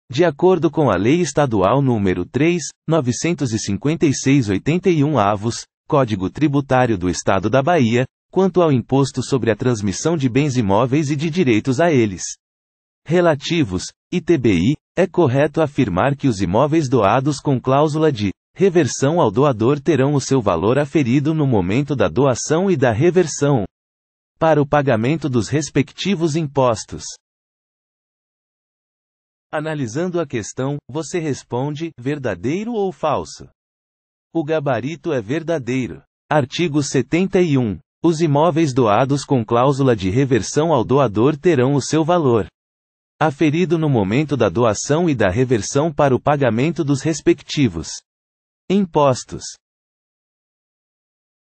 QScon Direito: [Art. 71, CT-BA] Texto de Lei
Art. 71, CTBA | Texto de Lei

De acordo com a Lei Estadual número 3.956/81 (Código Tributário do Estado da Bahia),. quanto ao Imposto sobre a transmissão de bens imóveis e de direitos a eles. relativos (ITBI), é correto afirmar que os imóveis doados com cláusula de. reversão ao doador terão o seu valor aferido no momento da doação e da reversão. para o pagamento dos respectivos impostos.

Gabarito: Verdadeiro